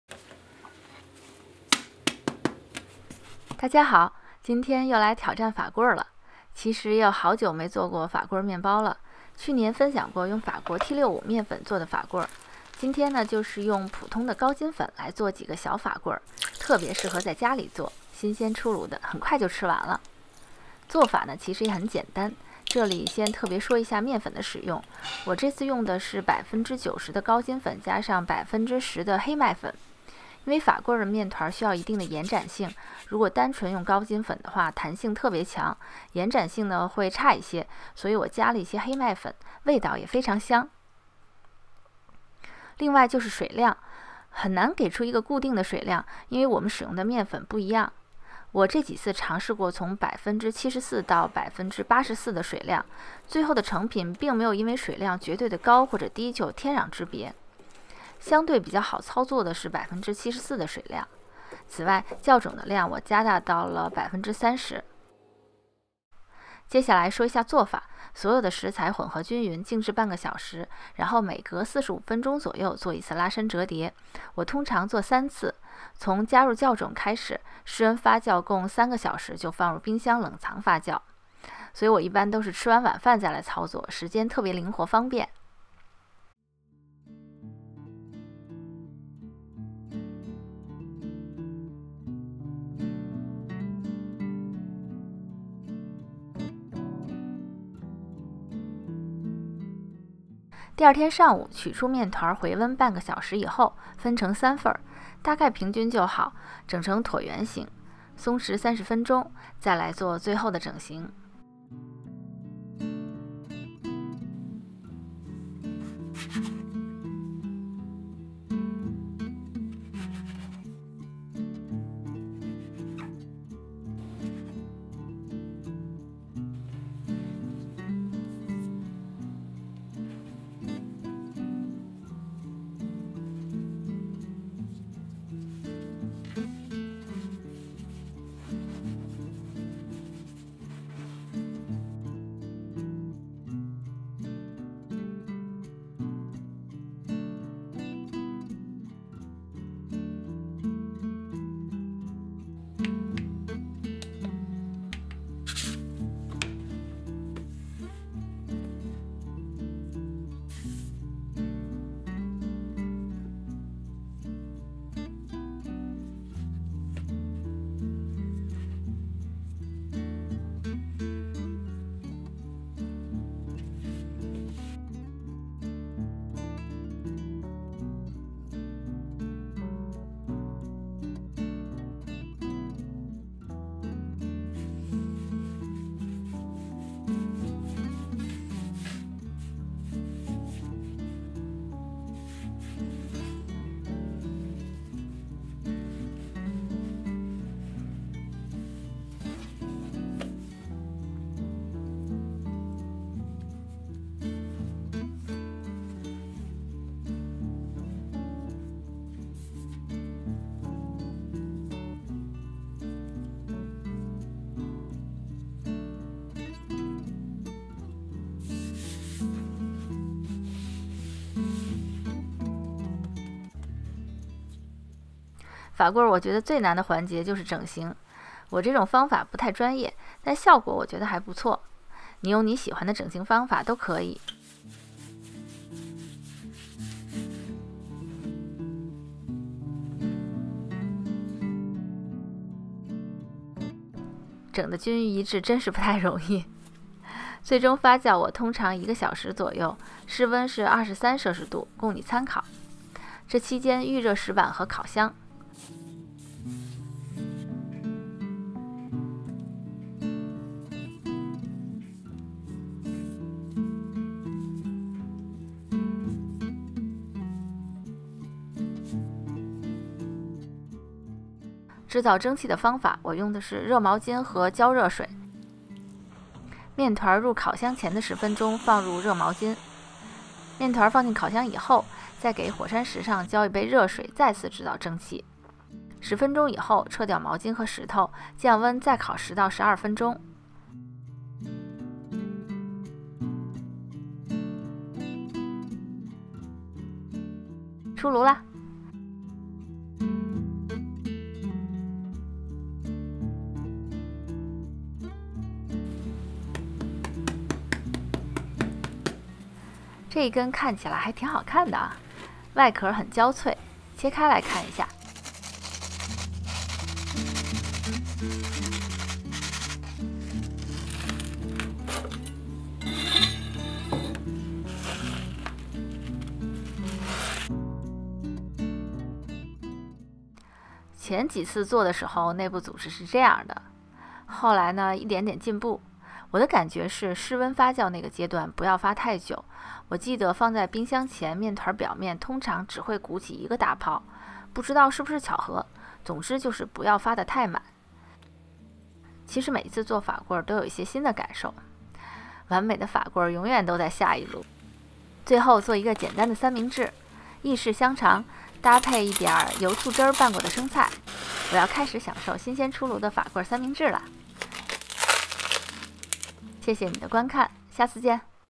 家庭小法棍 Homemade Sourdough Demi Baguettes
配方：

高筋粉 bread flour 225g
黑麦粉 rye flour 25g
水 water 185g
酵头 levain 75g
盐 salt 5g

去年用T65面粉做了很好吃的法棍，今年面粉也不好买。
就用家里的高筋粉加一点黑麦粉做了小法棍，大小朋友都很喜欢吃。
和乡村面包那么大体积不太一样，法棍的外壳占比很大，
如何做得皮薄而焦脆，同时内里又多洞而蓬松，
另外用纯鲁邦种，发酵不太容易掌控。视频里会分享一些我的实践体会：）
在家做法棍，吃的就是一个新鲜出炉，没有比这个更让人越挫越勇的了！